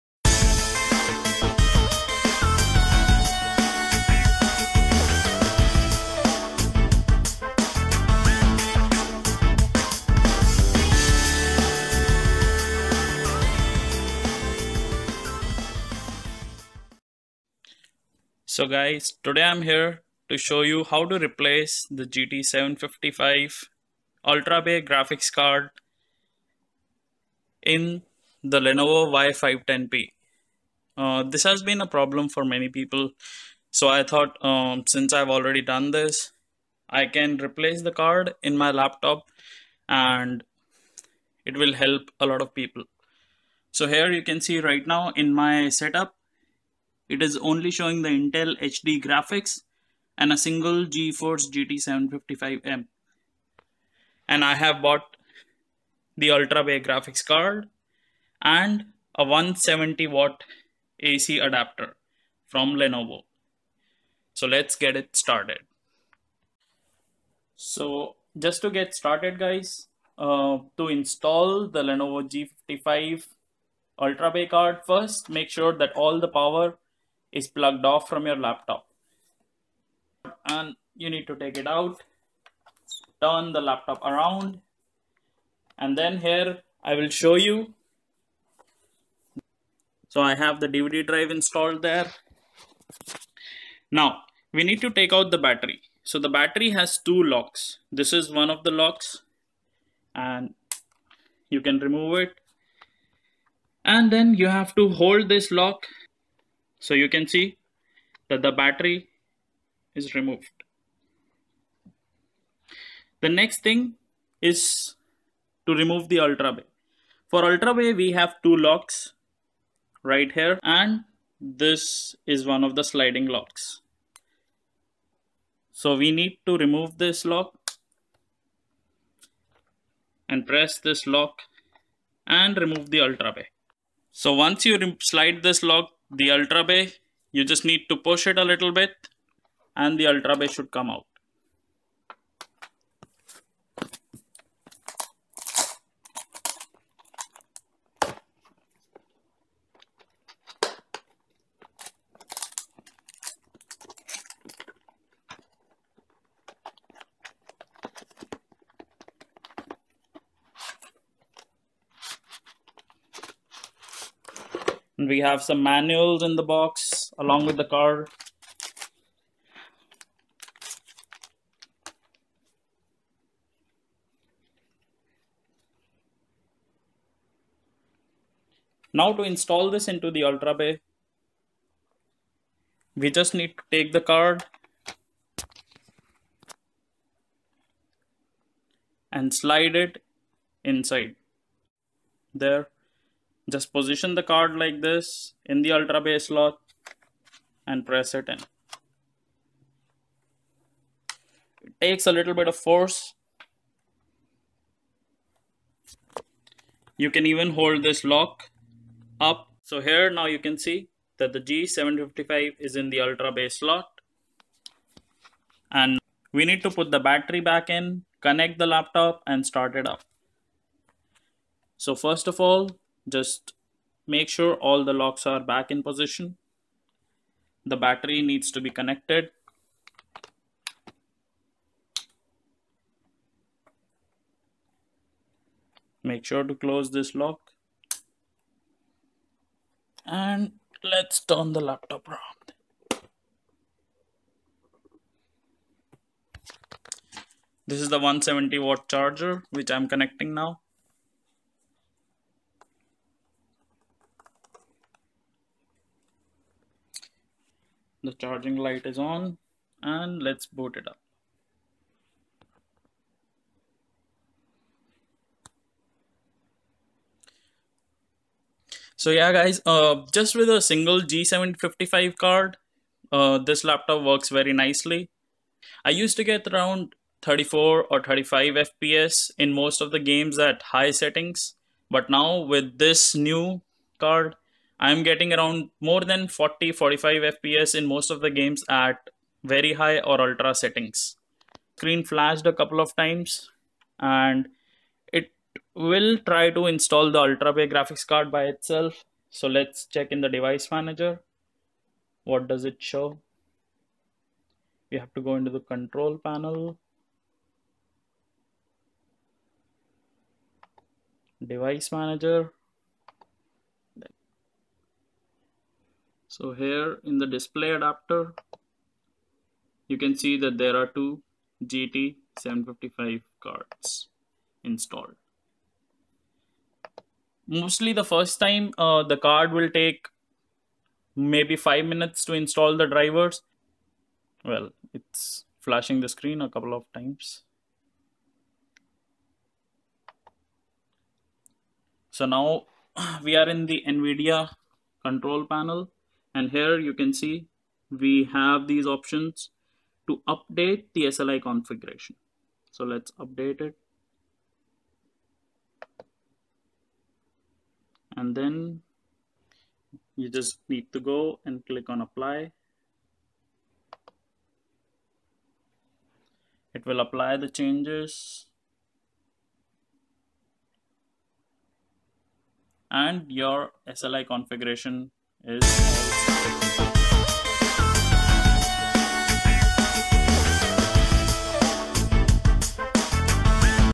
Replace Ultrabay Y510P graphics card
Hi Guys,

This video will demonstrate how to place and install your Ultrabay GT755M graphics card in the Lenovo Y510P.

My system already had Win 8.1 installed, so I did not need any VBIOS update from Lenovo website. If you have upgraded from Win 8, then after installing the card, please update the VBIOS as well. The link for the VBIOS is -

Please let me know in comments section if you have any questions or would like more info. This is my first video.
Thanks.